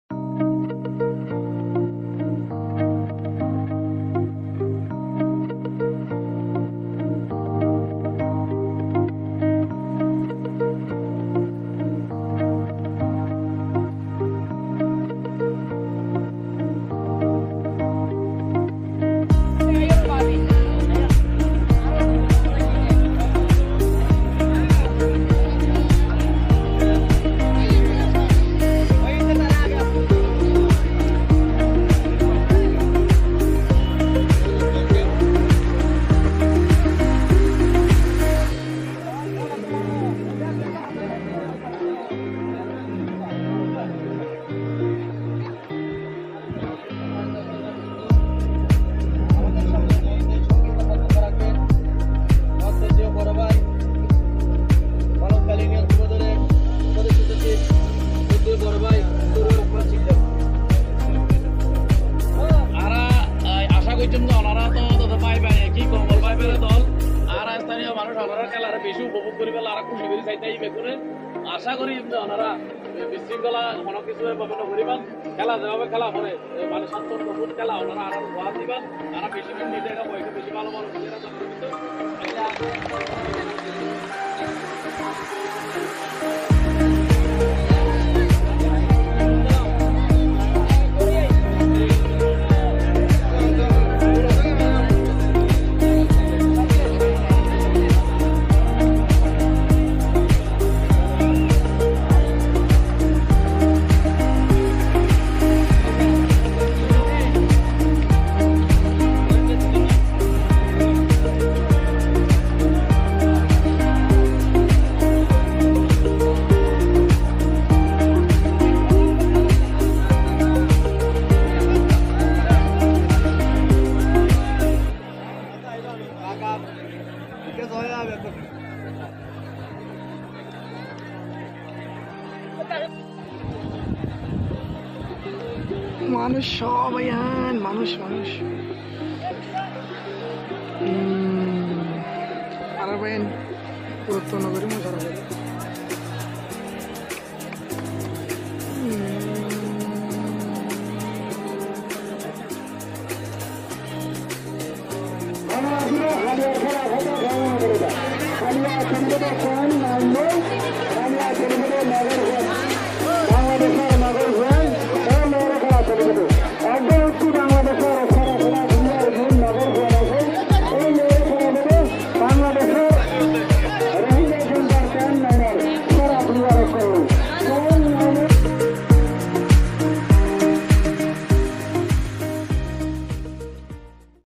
ASP Vs Gorah Khali were played well, Full time ended by 00=00
ASP Vs Gorah Khali were played well,
Full time ended by 00=00